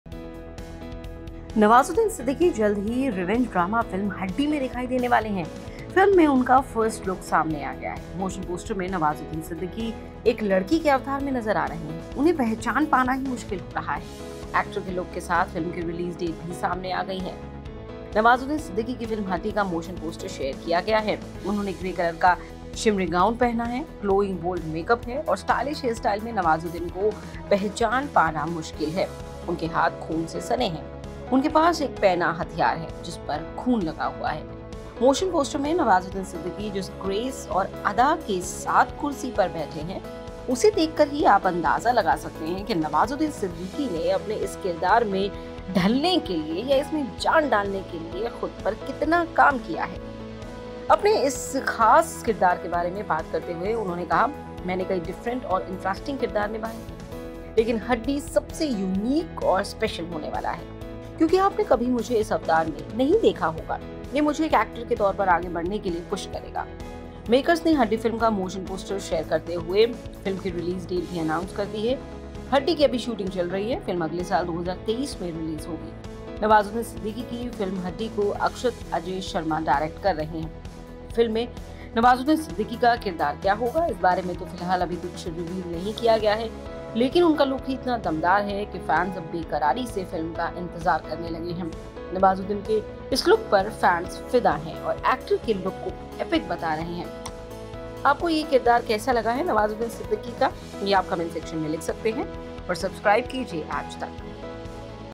नवाजुद्दीन सिद्दीकी अब बन गये है लड़की. Nawazuddin Siddiqui| Bollywood| Haddi| Movie
नवाजुद्दीन सिद्दीकी की फिल्म हड्डी का मोशन पोस्टर शेयर कर दिया गया है.
उन्होंने ग्रे कलर का शिमरी गाउन पहना हुआ है. ग्लोइंग बोल्ड मेकअप और स्टाइलिश हेयरस्टाइल में नवाजुद्दीन को पहचान पाना भी मुश्कि.पाना भी मुश्किल हो रहा है. उनके हाथ खून से सने हैं और उनके पास एक पैना हथियार रखा है, जिस पर खून लगा हुआ है. मोशन पोस्ट में नवाजुद्दीन सिद्दीकी जिस ग्रेस और अदा के साथ चेयर पर बैठे हैं, उसे देखकर ही आप अंदाजा लगा सकते हैं कि नवाजुद्दीन सिद्दीकी ने अपने  इस किरदार में ढलने के लिए खुद पर कितना काम किया है
The motion poster of Nawazuddin Siddiqui's film Bone has been shared.
She is wearing a gray shimmery gown. It is also getting difficult to recognize Nawazuddin in glowing bold makeup and stylish hairstyle. His hands are stained with blood and he holds a sharp weapon with blood on it. Seeing the grace and grace with which Nawazuddin Siddiqui is sitting on the chair in the motion post, you can guess how much work Nawazuddin Siddiqui has worked on himself to get into this character.